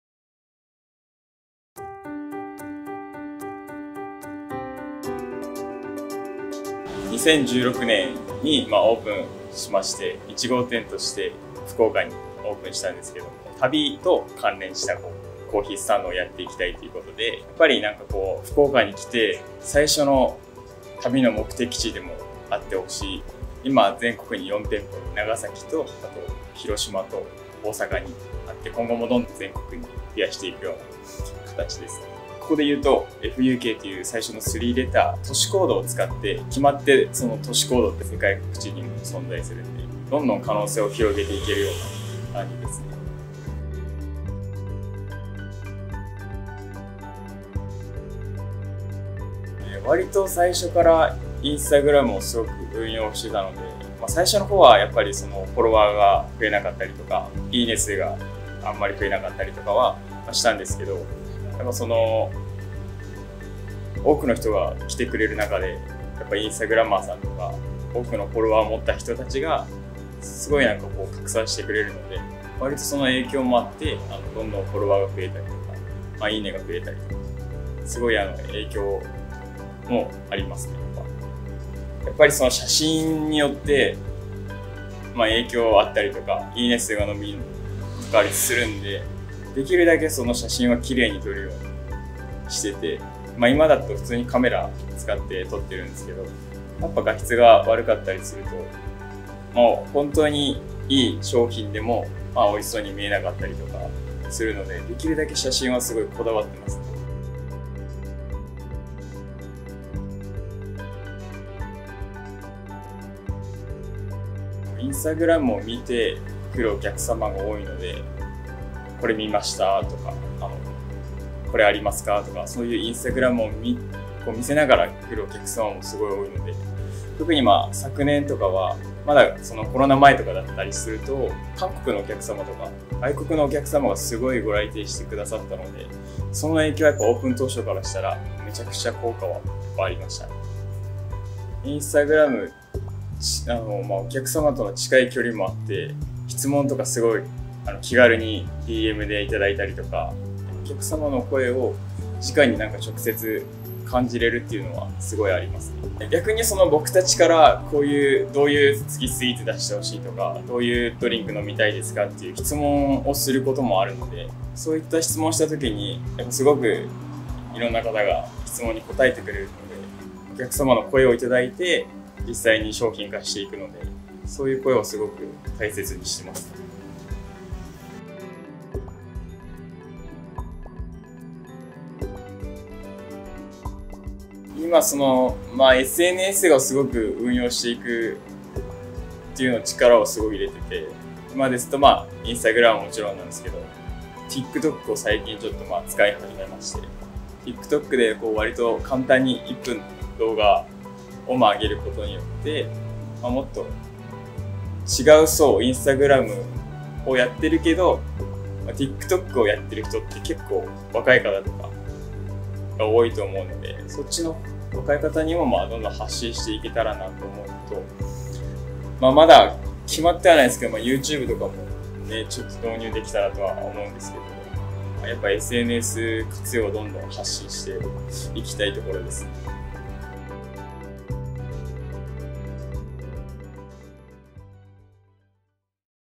ここからアプリ アプリ導入事例動画【FUK COFFEE】
FUK COFFEE（フックコーヒー）は2016年に福岡に1号店をオープンしたコーヒースタンドです。名前のFUKは福岡空港の空港コードから名づけました。このコーヒースタンドは、旅行・空港・飛行機をテーマにしており、福岡の他にも、長崎（NGS）、大阪（OSA）、広島（HIJ）と、現在では全国で4店舗を展開しています。今後も全国に店舗展開を検討する中で、SNSを活用してお店の認知度を上げ、集客を増やしています。